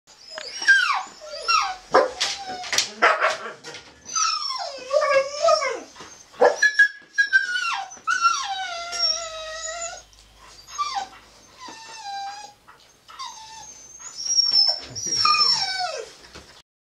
Siberian Husky finds his own crate
1 month old Siberian walking